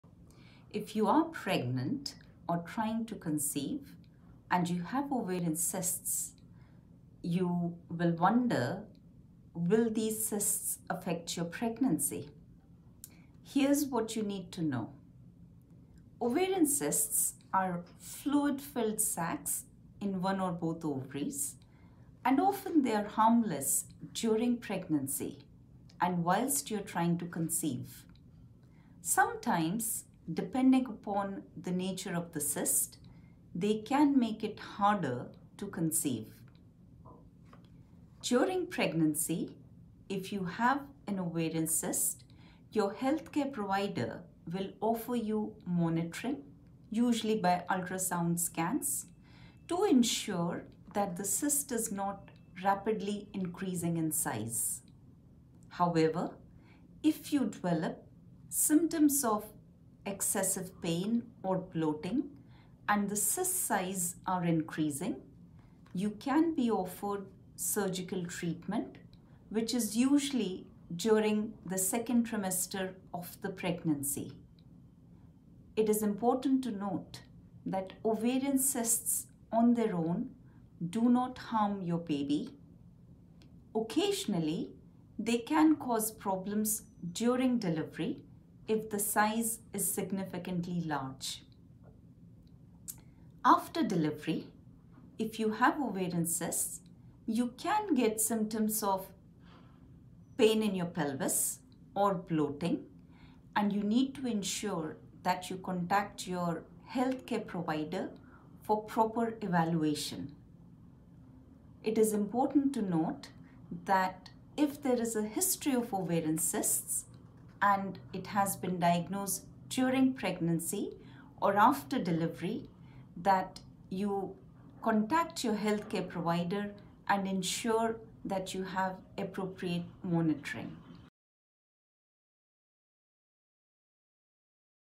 Pregnancy and ovarian cysts | UHL NHS Trust
Consultant Gynaecologist, Neelam Potdar, explains about ovarian cysts during pregnancy.